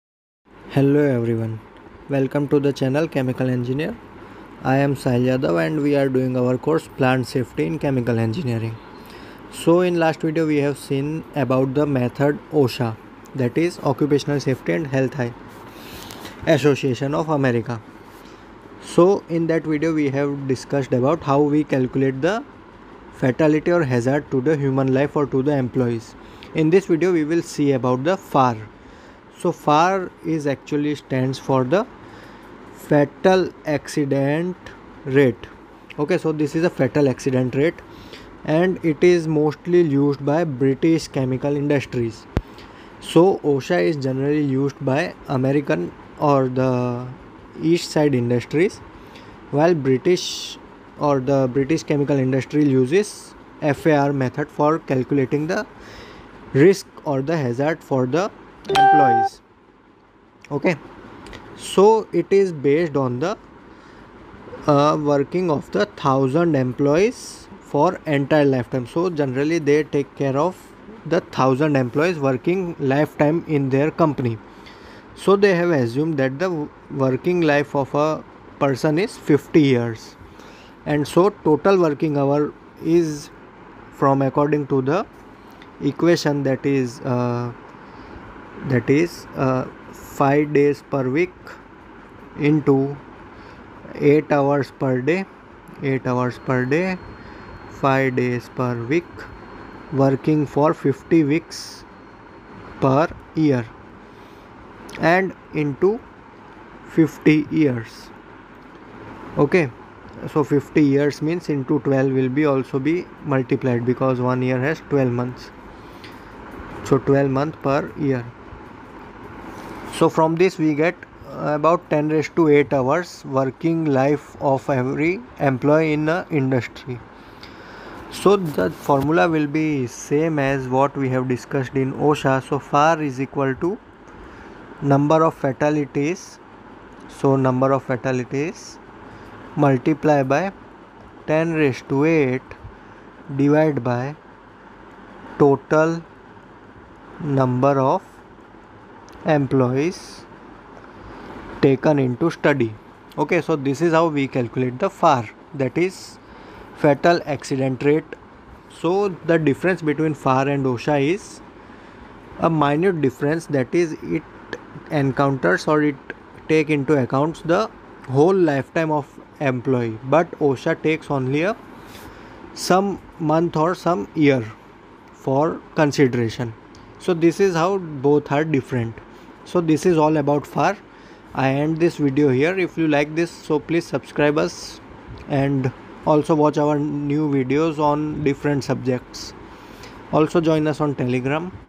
Fatality Accident Rate - FAR | Plant Safety | Chemical Engineer
Topic covered :
Plant safety : Fatality accident rate(FAR)
This is a method for calculating the hazard related to workers due to exposure of different hazardous materials in industries.